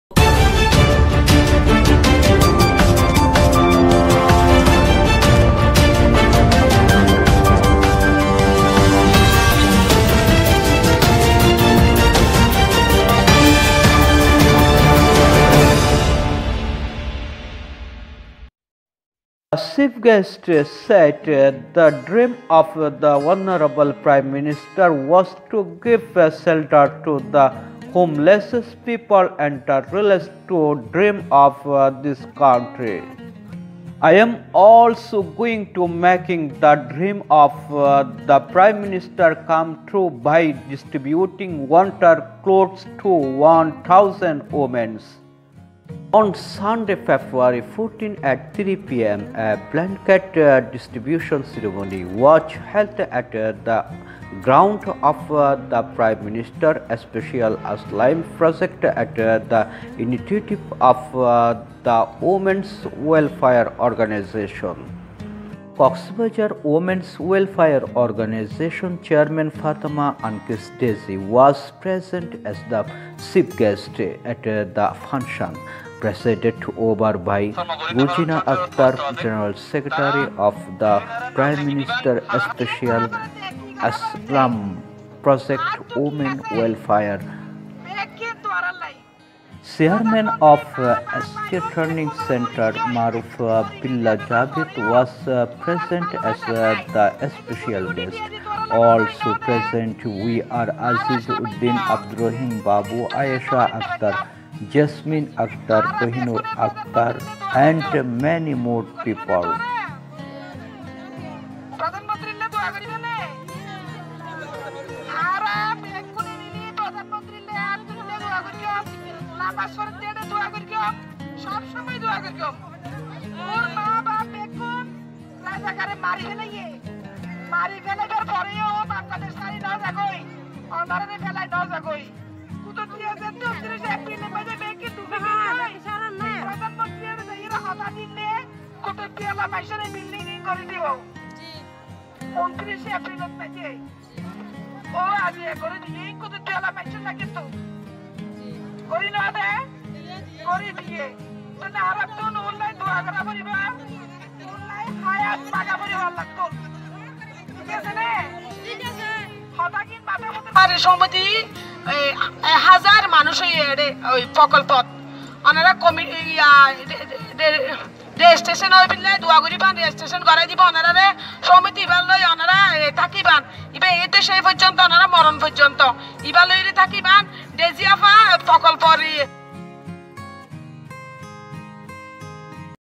distribution ceremony
Distribution of blankets at Khurushkul Shelter Center on the initiative of Women’s welfare organisation.The chairman fatama dizy of Cox's Bazar Women's Welfare Association was present as the chief guest at the blanket distribution function.